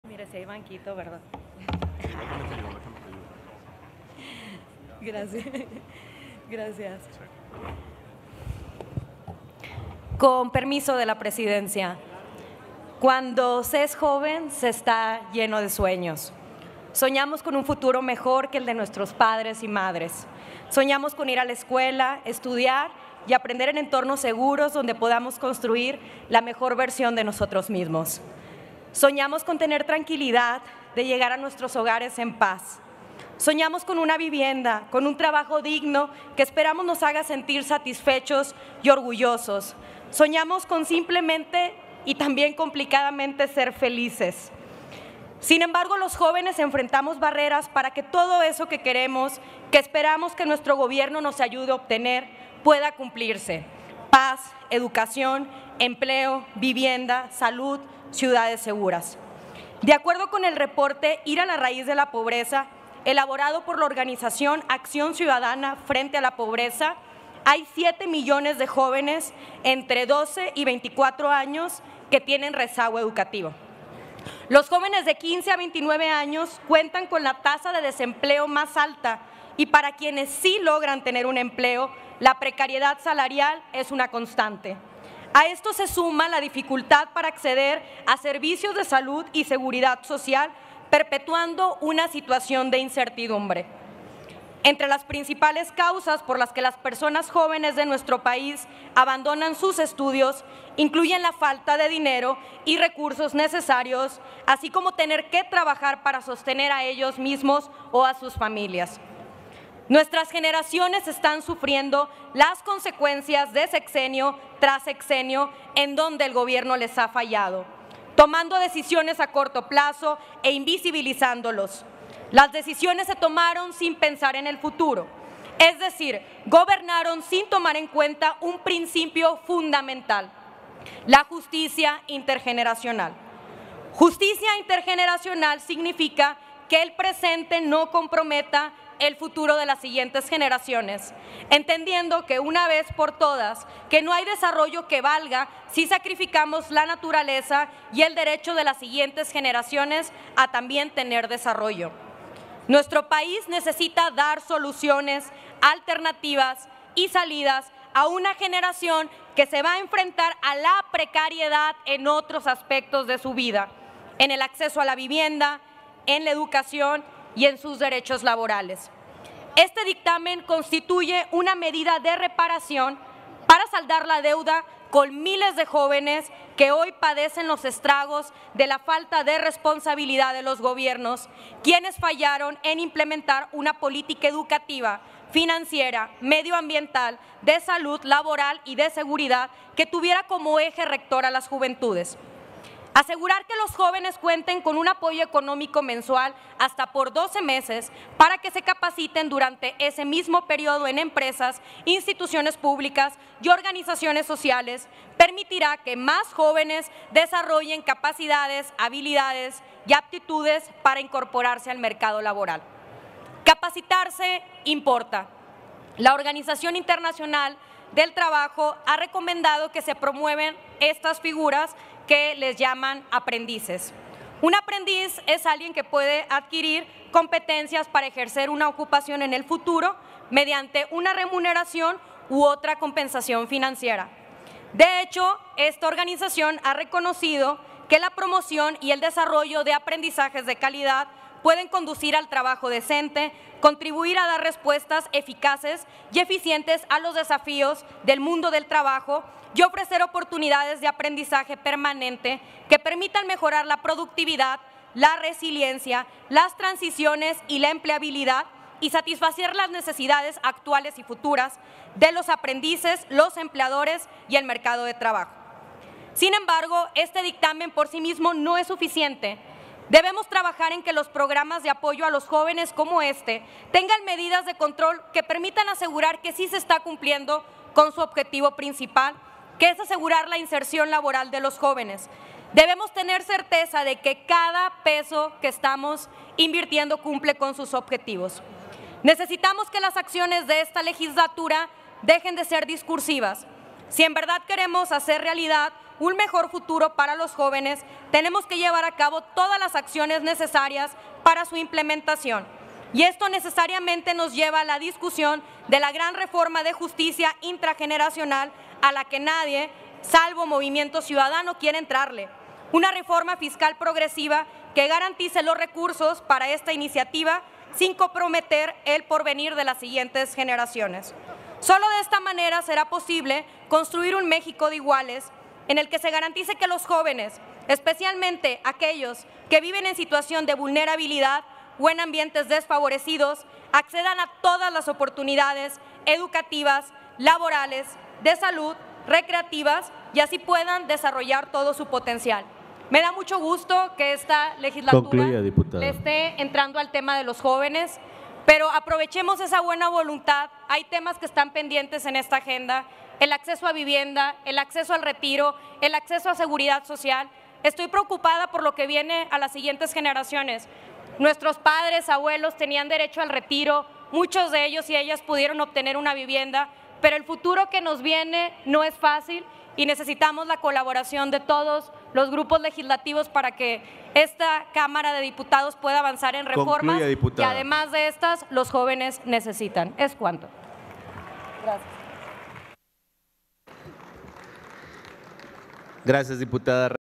Dictamen Jóvenes construyendo el futuro | Iraís Reyes
En la BancadaNaranja sabemos que no hay desarrollo que valga si sacrificamos la naturaleza y comprometemos el futuro de las siguientes generaciones.

Necesitamos alternativas y soluciones para evitar que los jóvenes viven en la precariedad. Así lo dijo Iraís Reyes.